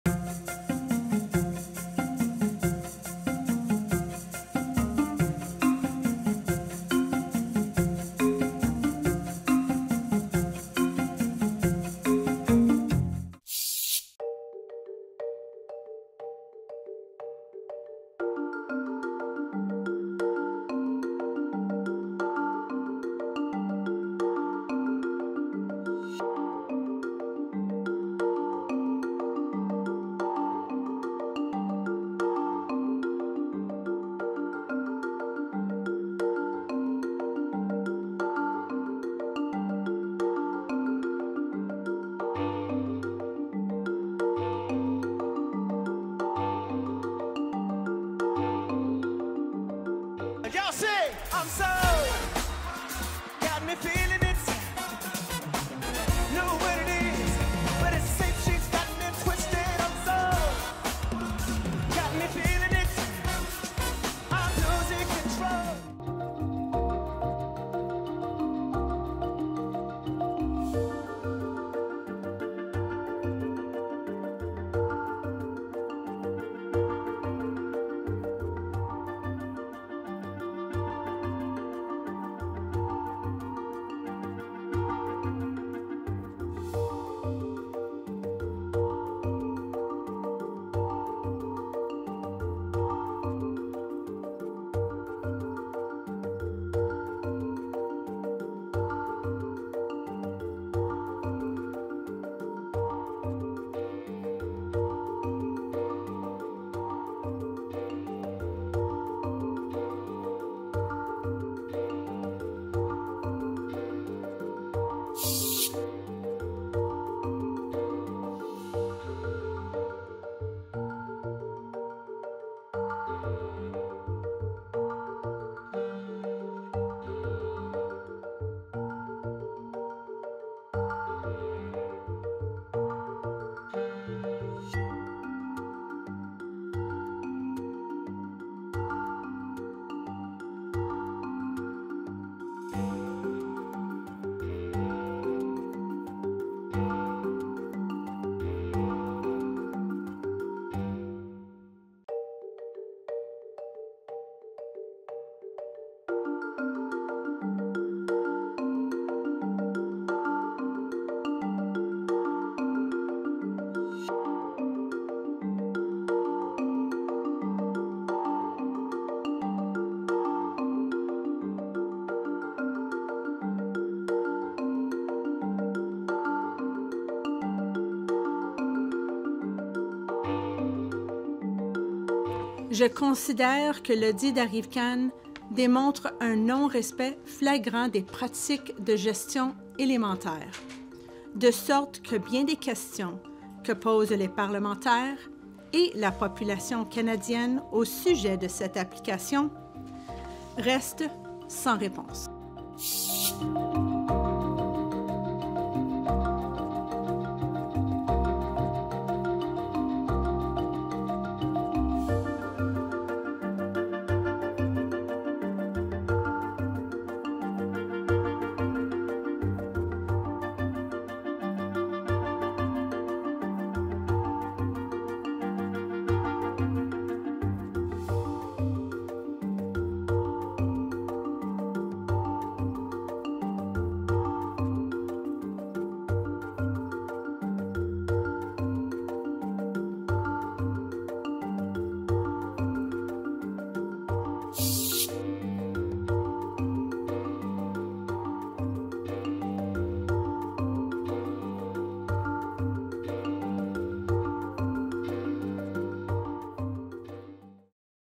Les nouvelles en LSQ du 16 février 2024
Cette semaine, l’ASL au Super Bowl, le prix exorbitant de l’application ArriveCan et le drame survenu au Manitoba.